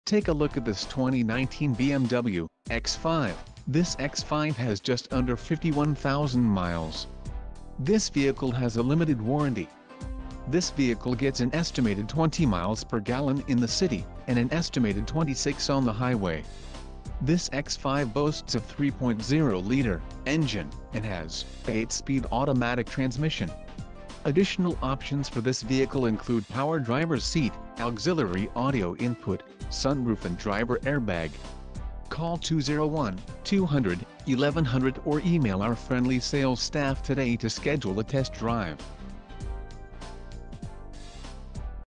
2019 BMW X5 - Jersey City, NJ
New Jersey State Auto Used Cars, Jersey City, NJ - BMW X5 xDrive40i Sports Activity Leather Sunroof Navigation Reverse Camera